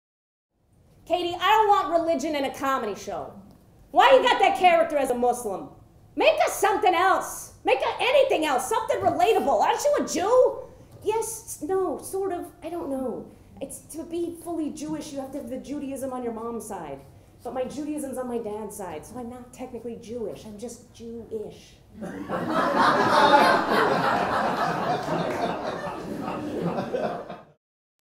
Why I Died, a Comedy! - Stan and Katie. "Aren't you a Jew?"
When the pressure of being human becomes too much, we should never forget that spiritual suicide is always an option! If it's true that our attachment to worldly concerns and ambitions is the cause of our suffering, then dissolving those ambitions should bring complete and perfect peace to our hearts, right? Maybe...

Audience favorite, and Nationally touring Solo Artist and Comedienne Katie Rubin premiers her fourth and most recent show. "Why I Died, a Comedy" explores the questions and challenges raised by the soul-sucking aspects of being a person among other people. Don't worry. You can handle the truth.